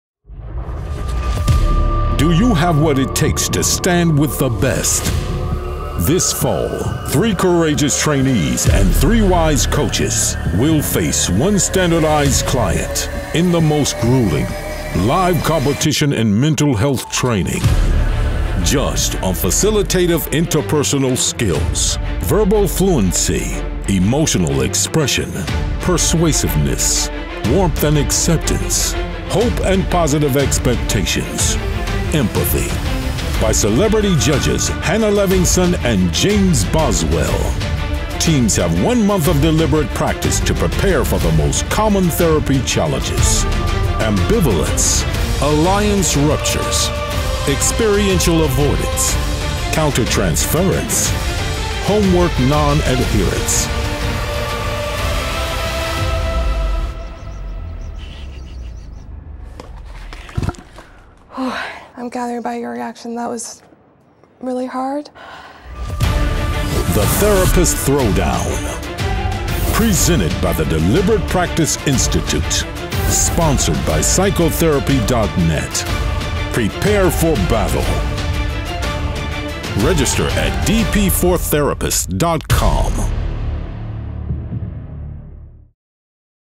Therapist Throwdown Promo Video
The Sentio University 20-month Marriage and Family Therapy MA program provides rigorous and intensive hybrid on-line and in-person training.  We are now enrolling students for our next cohort!
Join our email list to be alerted about new Sentio training opportunities for therapists, supervisors, faculty, and trainees
Sentio is a 501(c)(3) non-profit with a mission to advance the field of therapist training.